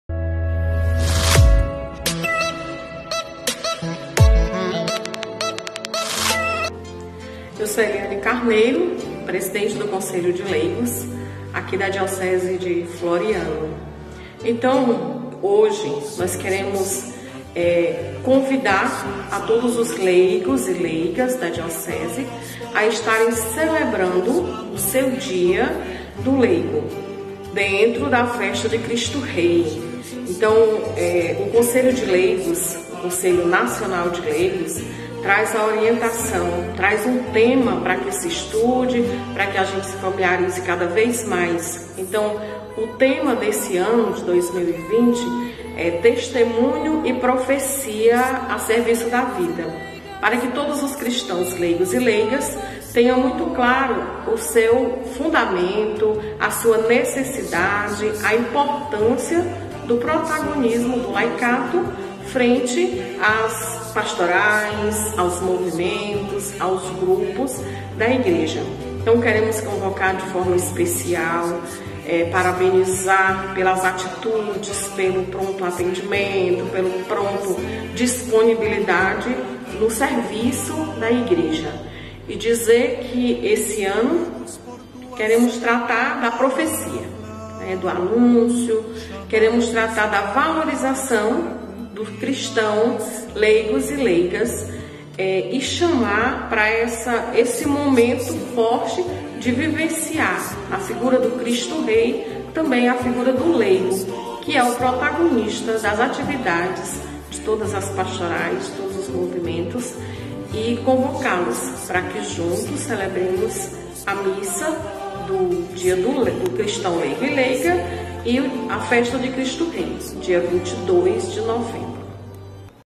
MENSAGEM PARA O DIA DO LEIGO E LEIGA - DIOCESE DE FLORIANO / NE 4 - PIAUÍ
Eco do Dia Nacional dos Cristãos Leigos e Leigas.
DIOCESE DE FLORIANO - Regional Nordeste 4 -Piauí.
Saudações da Presidente do Conselho de Leigos da Diocesano.
Hoje dia 22 de novembro é o Dia Nacional dos Cristãos Leigos e Leigas 2020.
A data é celebrada na Solenidade de Cristo Rei. Este ano, o lema é: Cristãos Leigos e Leigas: testemunho e profecia a serviço da vida e o lema: “Eu vos chamei a serviço da justiça” (Is 42,6).